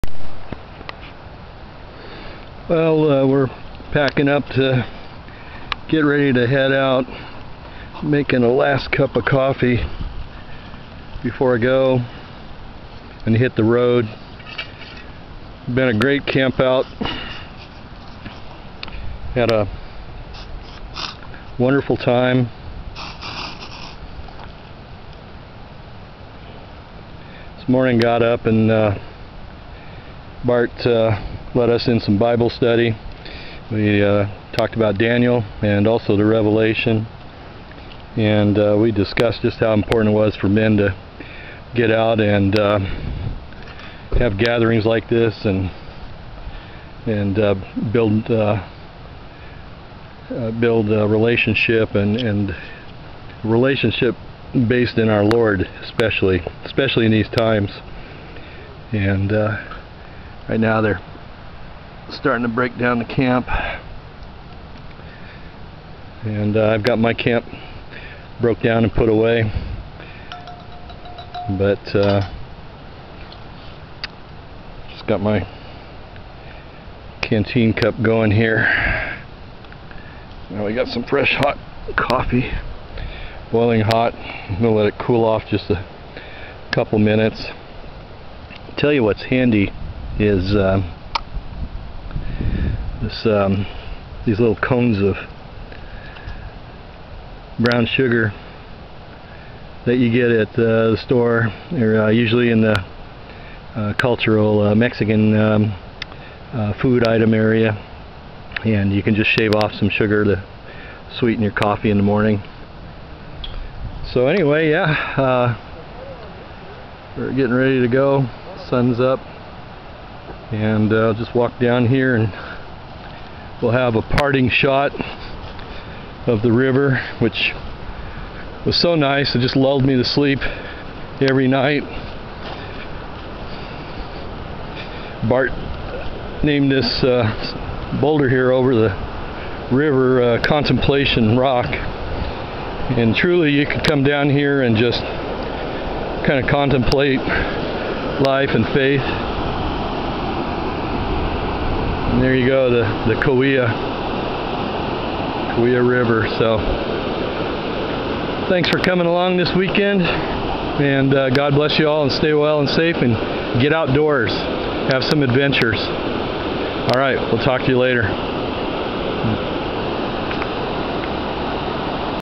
Morning coffee and Kephart-Nessmuk Brotherhood campout wrap-up
Coffee before breaking camp and heading home. It was a wonderful weekend, and I am already looking forward to the next gathering!